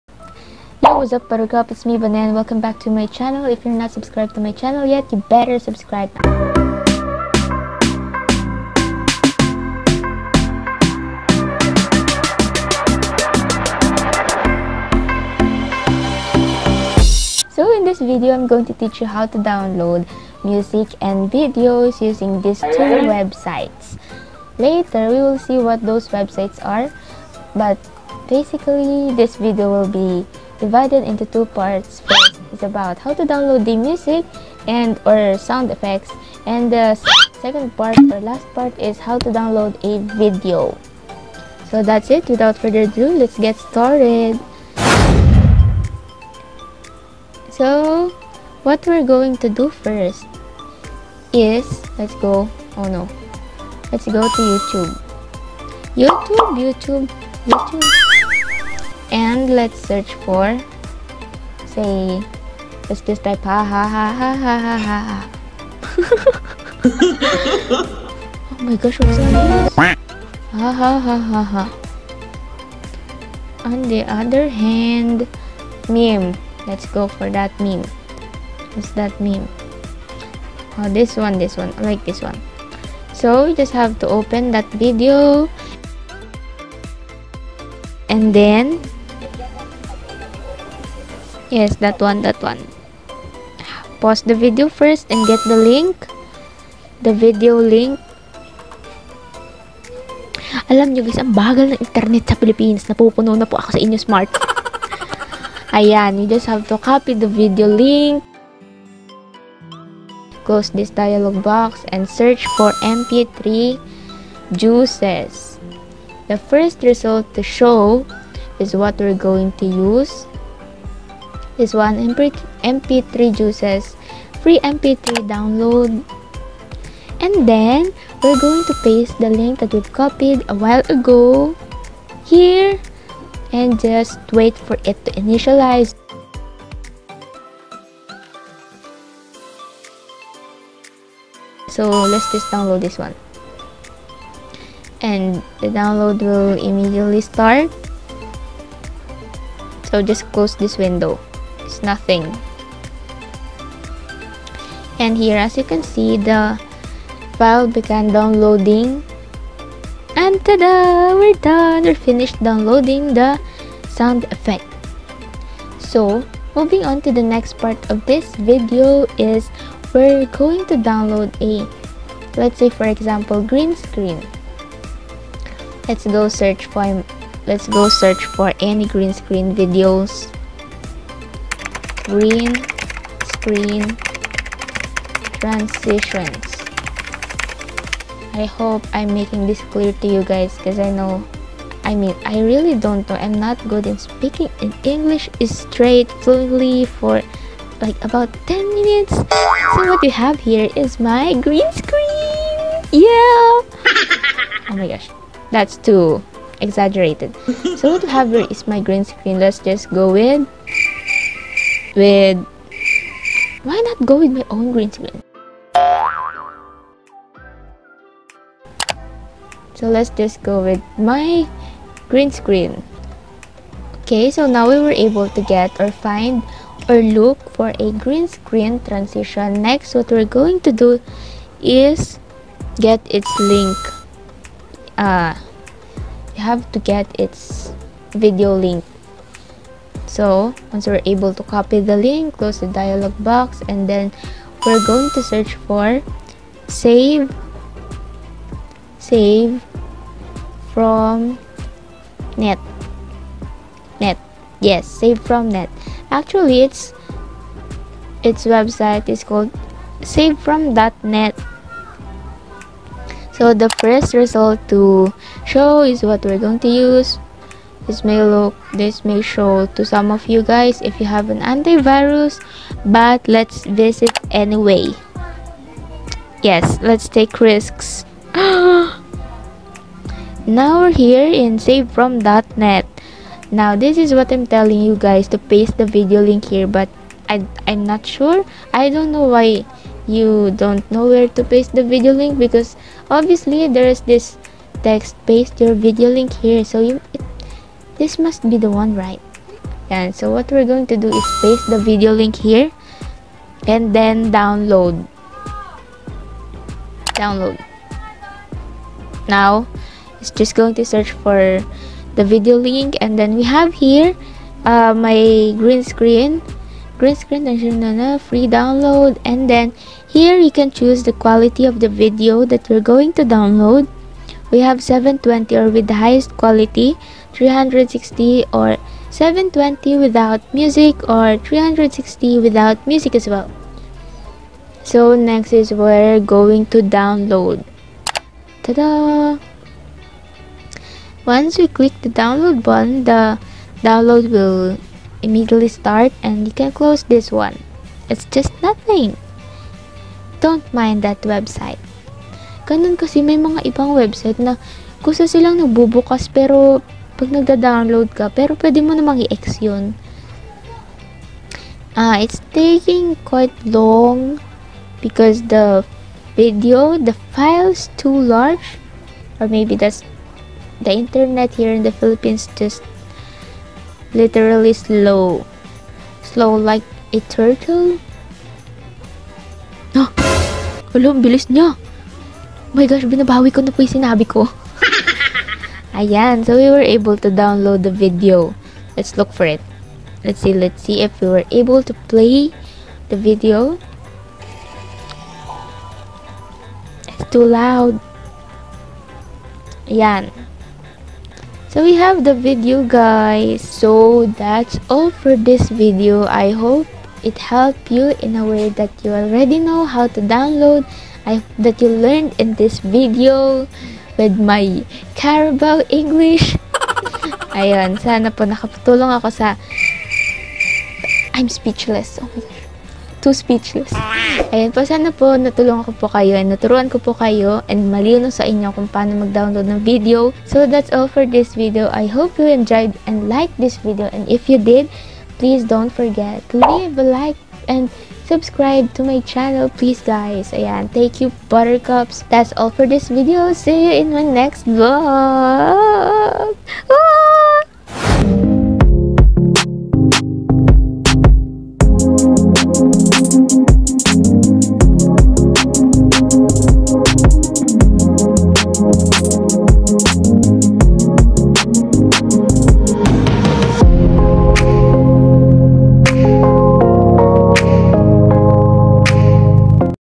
HOW TO DOWNLOAD AUDIO / VIDEO? ‖ by Bane Solomon
Hey what's up buttercup! It's me Bane and in this video you will learn how to download videos or audio for free.